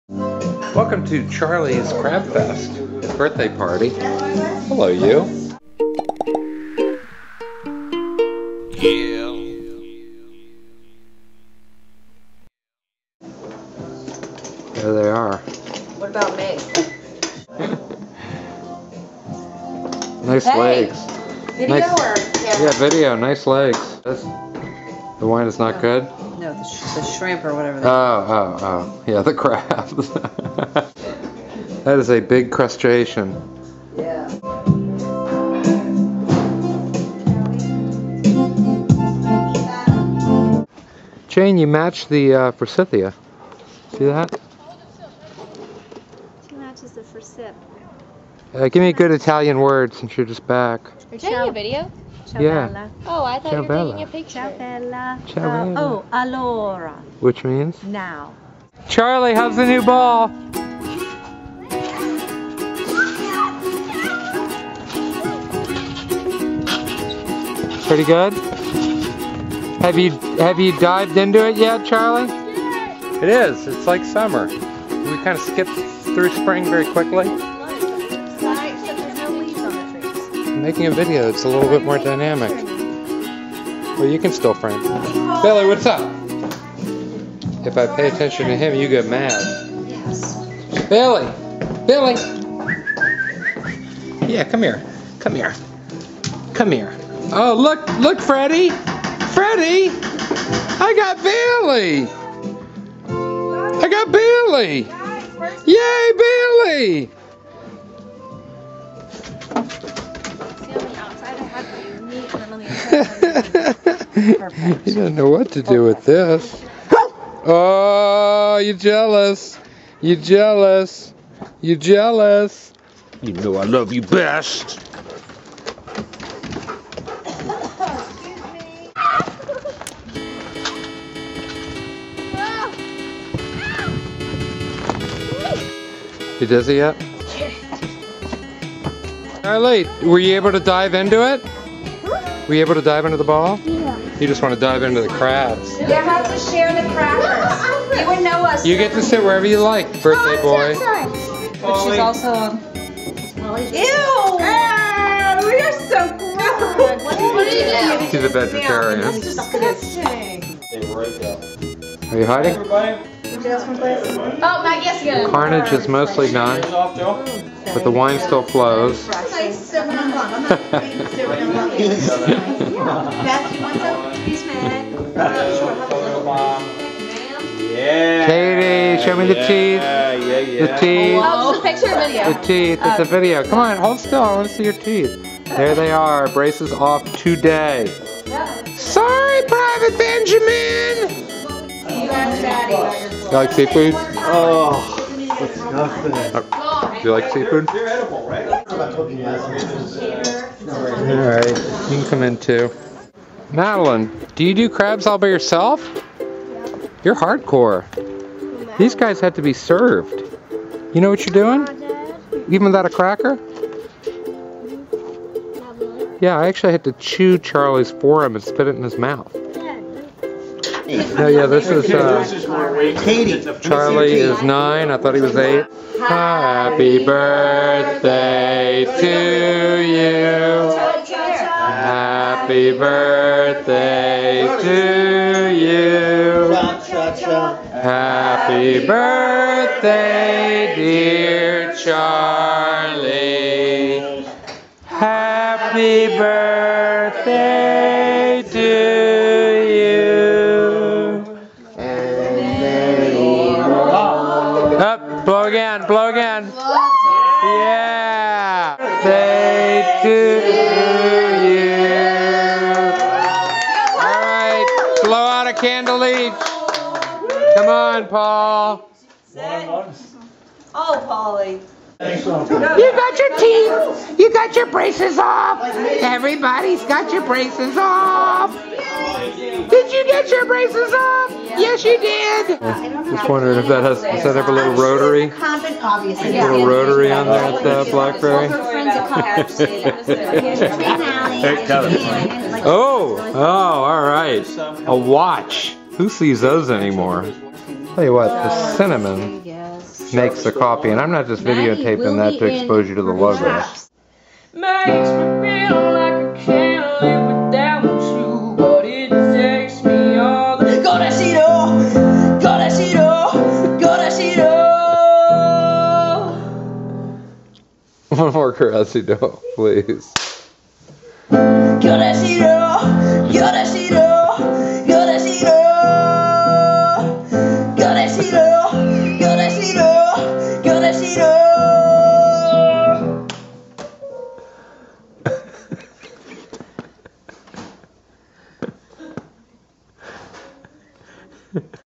Crabby Birthday
Charlie's 9th birthday is a crab festival. Katie gets her braces off. Patch plays us out.